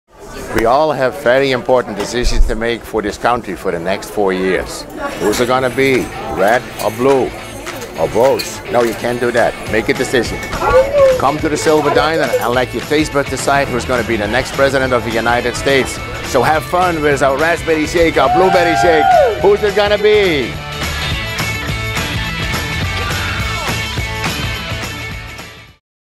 Shake the Vote at Silver Diner!
Let your taste buds decide the election and choose the Blueberry or Raspberry Shake! Check for daily updates!